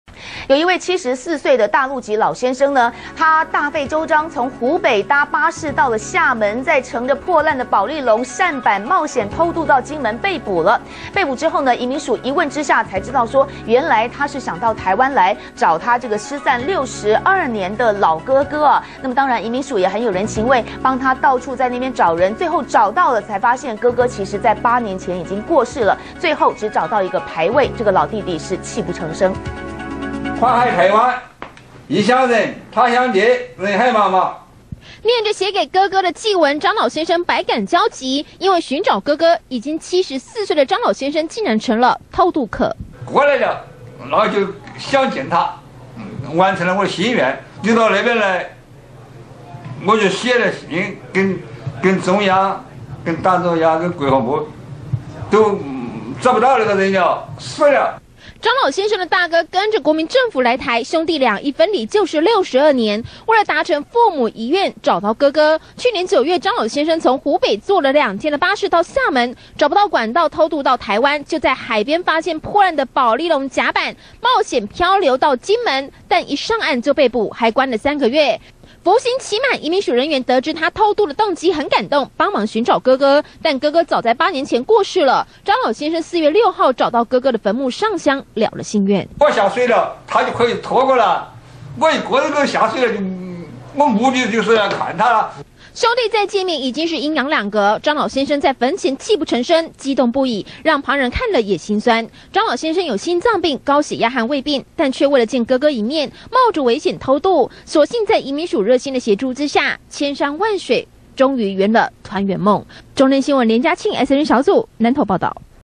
相隔62年！弟偷渡來台尋兄 只見到墓碑
中天新聞》一名74歲大陸偷渡犯張梅生日前坐保麗龍舢舨來台尋兄，遭移民署查獲。張事後得知分離一甲子兄長已過世，在遣送回陸前，移民署南投收容所6日特別安排他到五指山國軍公墓祭拜。張老朗讀祭文時嚎啕大哭，以最敬禮方式，表達對台灣的感謝。